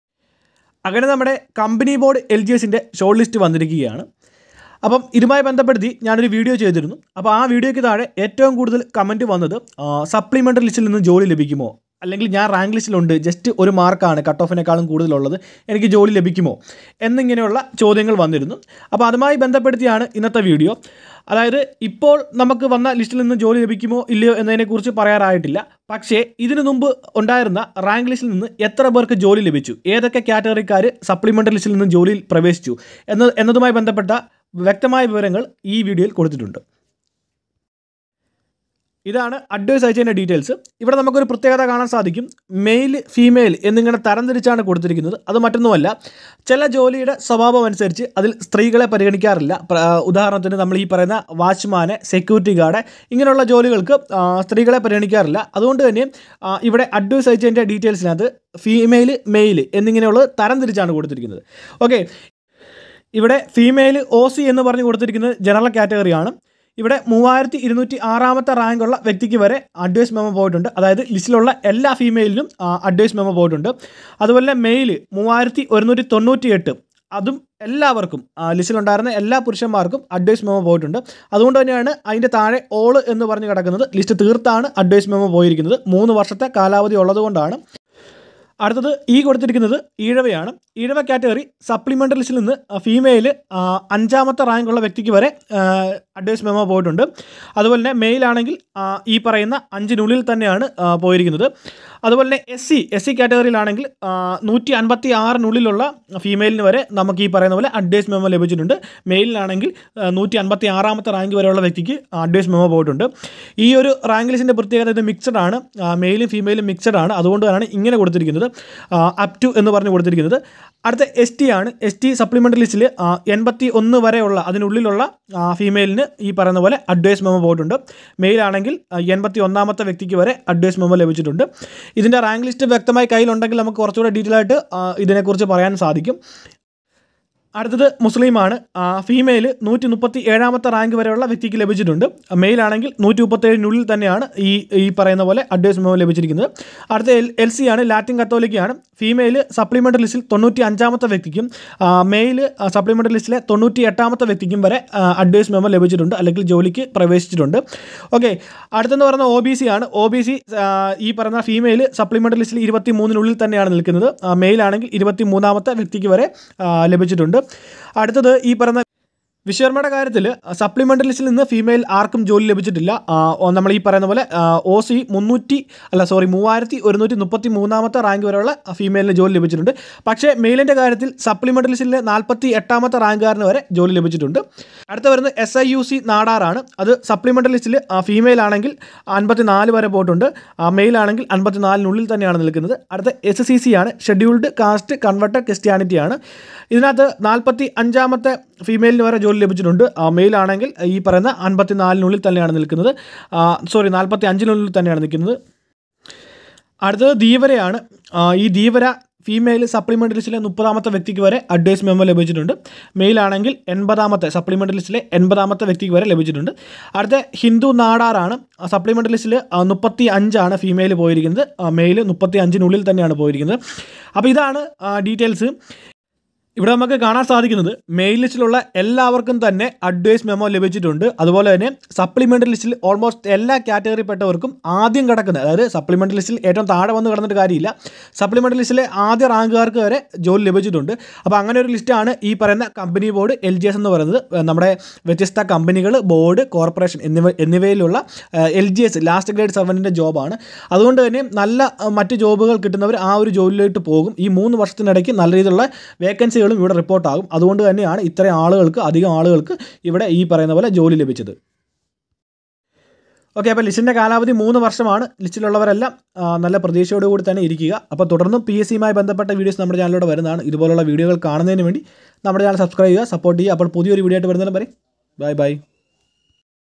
Supplementary List ൽ ഉള്ളവർക്കുവരെ ജോലിലഭിച്ചു 📌Company Board LGS 📌Previous Rank List Full Details
Company Board LGS Main ലിസ്റ്റിൽ വന്നാൽ ജോലി ഉറപ്പ്.. Supply ഉള്ളവർക്കും ജോലി